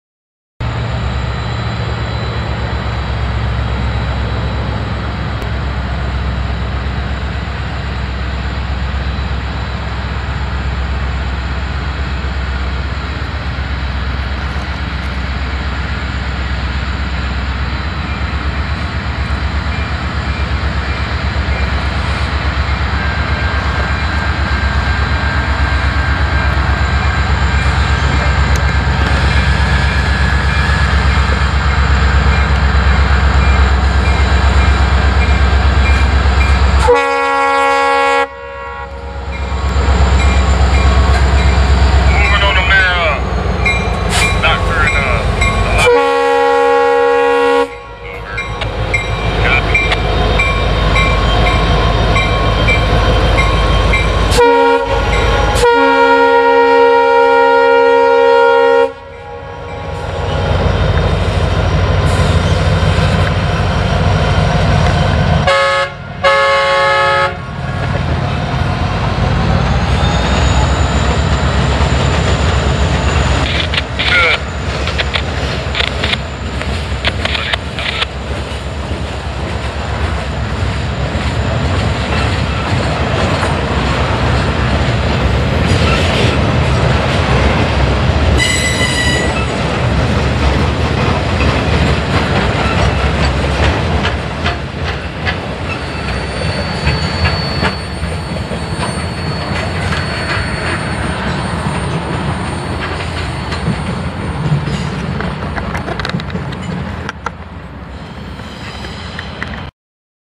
:HD: NS O55 Caterpillar Train Southbound
Norfolk Southern train O55 departs the siding in Bunch, GA on 03/30/2013 with an all Caterpillar load and a Christmas tree on the last car.

This was the third of four (4) trains to come through this area within the same hour. Trains 276 & 738 were northbound on the main, while trains O55 & 119 were stacked up in the same siding, waiting to go southbound. Order is: 276, 738, O55, 119.

NS 6768 - EMD SD60M

Video taken with a Canon Vixia HF R21